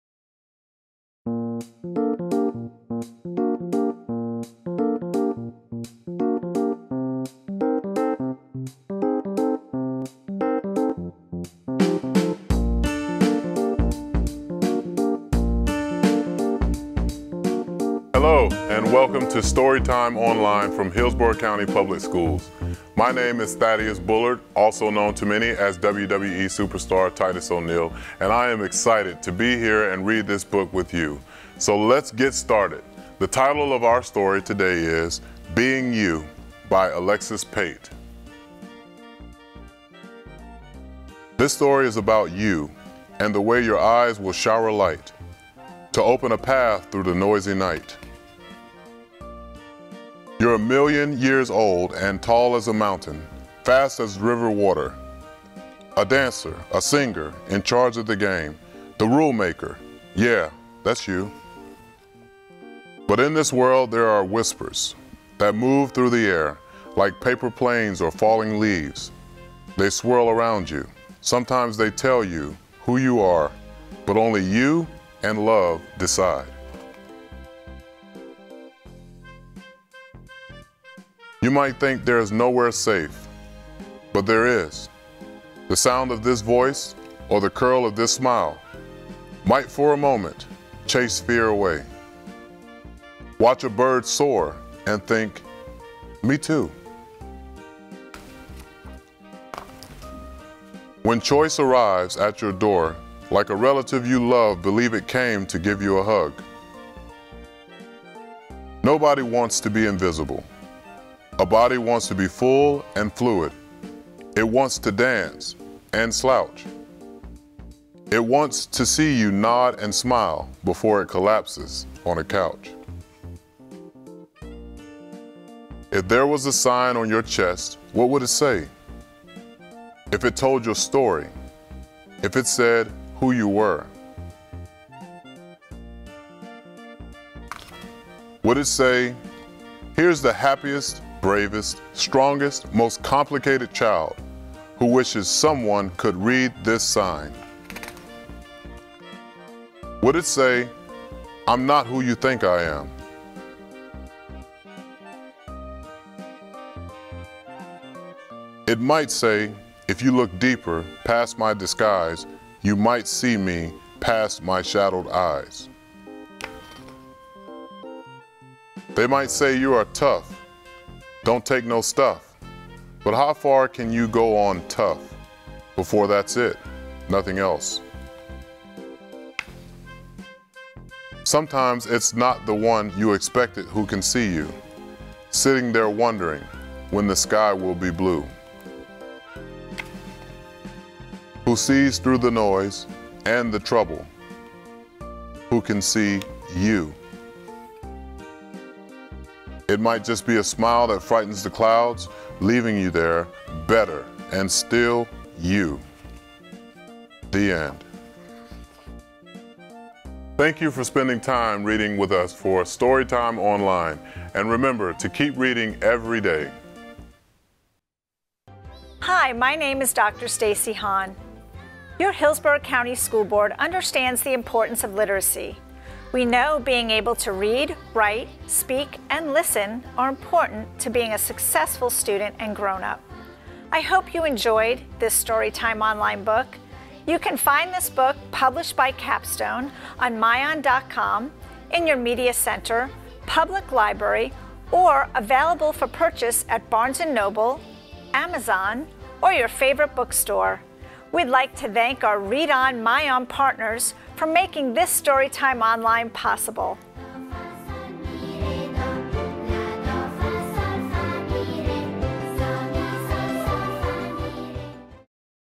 Storytime Online - Thaddeus "Titus O'Neil" Bullard reads Being You
Join WWE Superstar Thaddeus "Titus O'Neil" Bullard as he reads "Being You" for Storytime Online from Hillsborough County Public Schools.